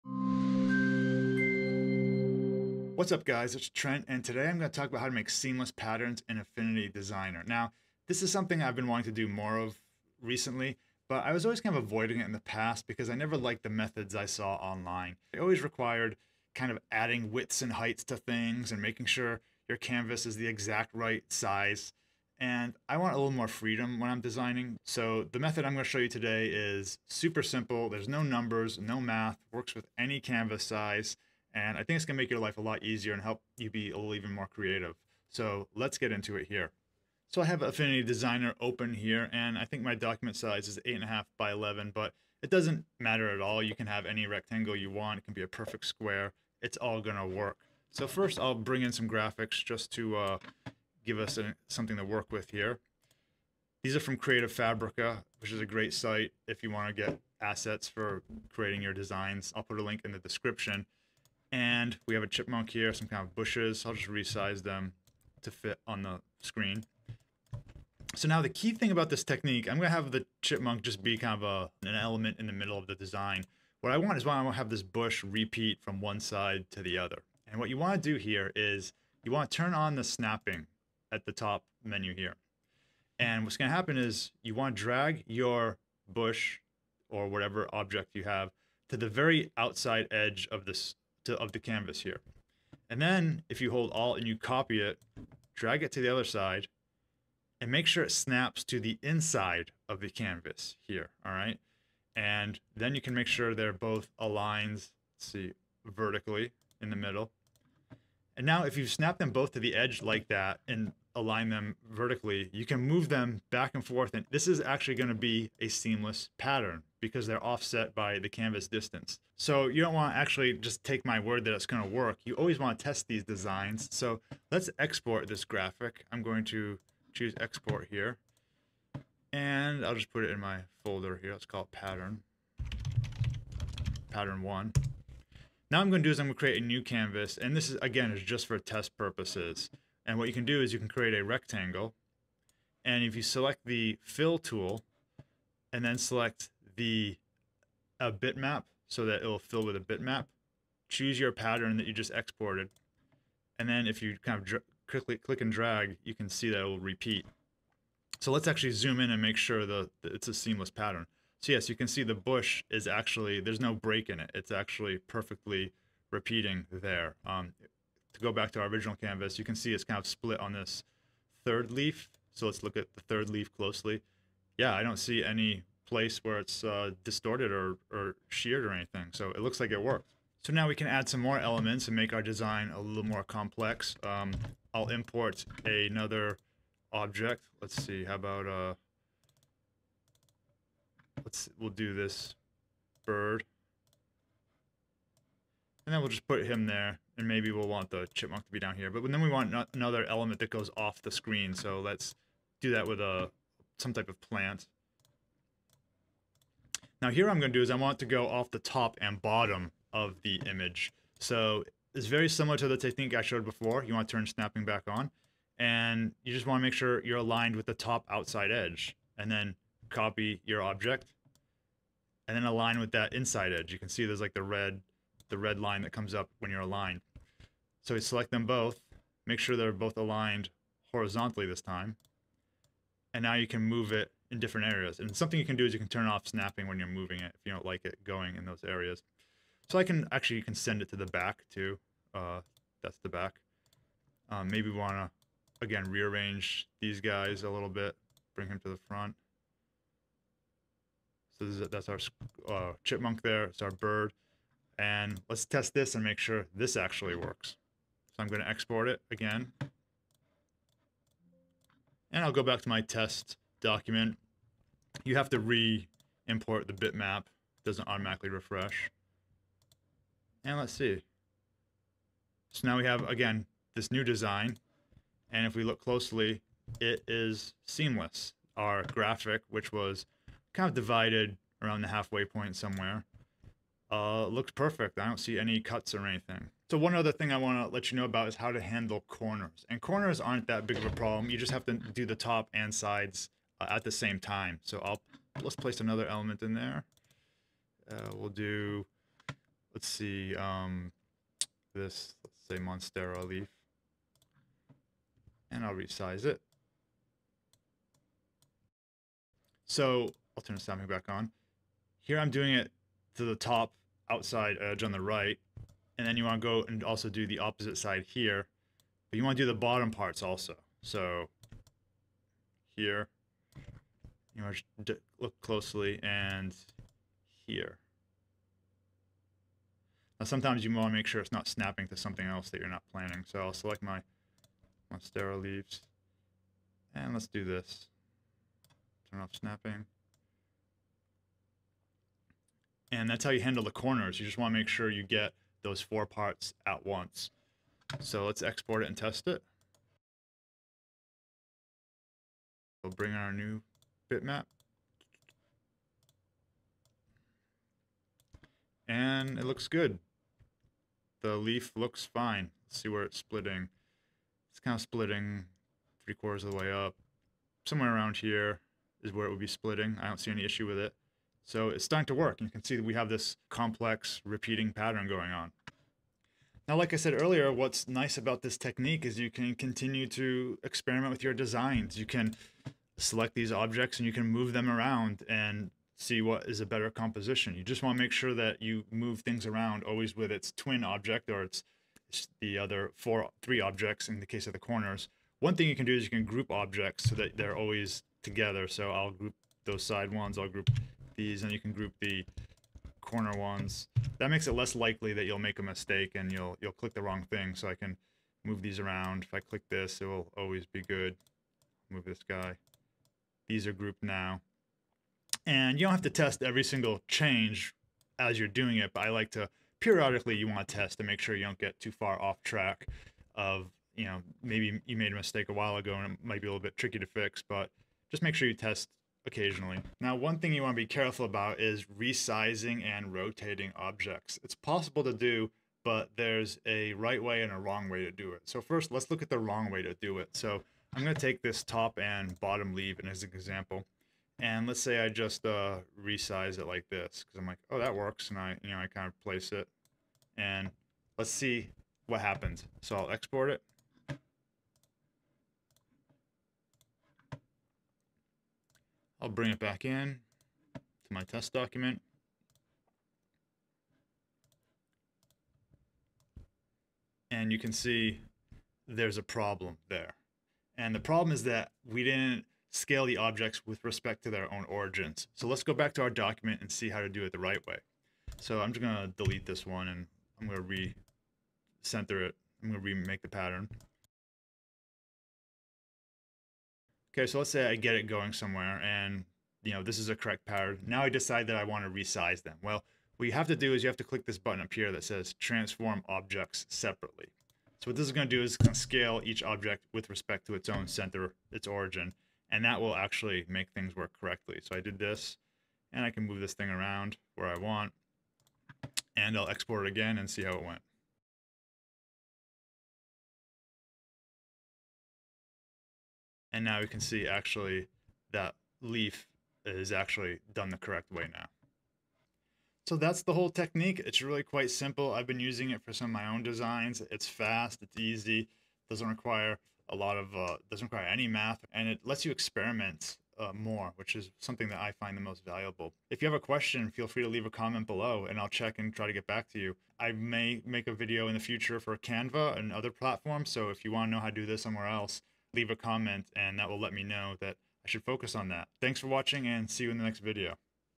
Design Seamless Patterns Like a Pro : Easy Step-by-Step Tutorial for Affinity Designer
No math! Any canvas size! Easy to edit! In this step-by-step tutorial, I’ll show you how to create seamless patterns like a pro using the powerful design software, Affinity Designer. Whether you're a beginner or an experienced designer, this tutorial will guide you through the entire process.

00:00 Intro
00:37 Left-and-Right Seamless Pattern
03:00 Top-and-Bottom Seamless Pattern
04:46 Corner Seamless Patterns
07:10 Scaling : Be Careful!
08:40 Final Result